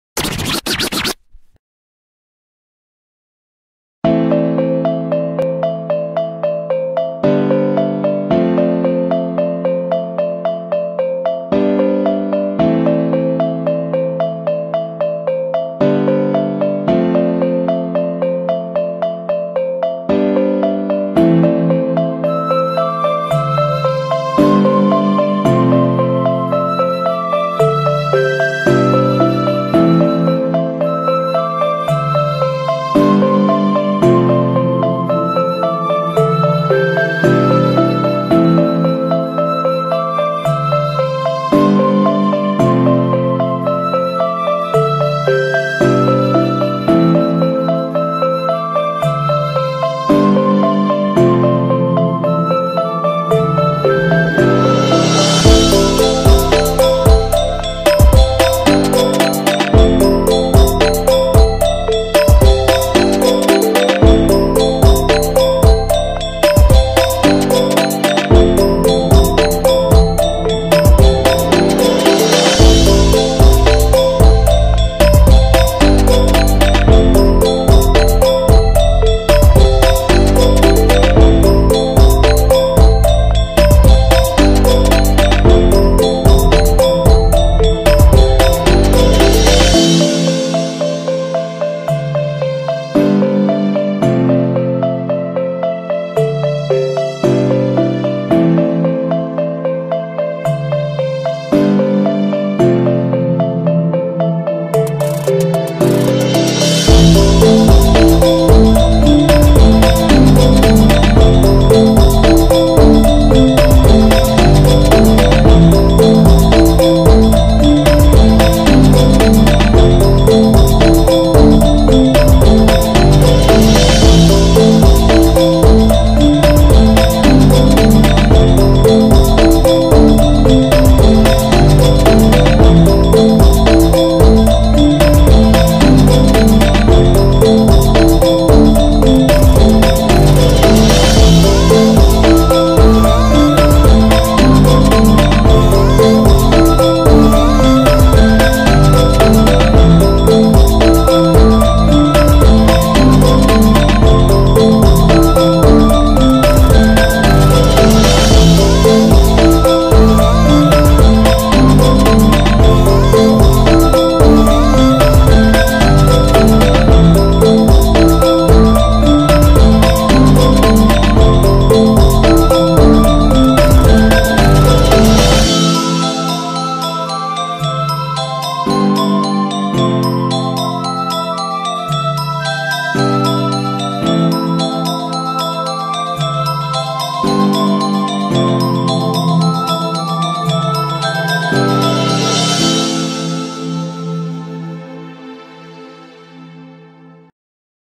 Indonesian Music Ethnic/Culture #14 - Gamelan + Suling Modern EDM
Cinematic Backsound Music bernuansa etnik nusantara, dengan paduan instrument tradisional yang indah.

Sebelumnya Saya ucapkan terima kasih kepada yang sudah menonton video ini. Semoga video ini bermanfaat bagi teman – teman sekalian.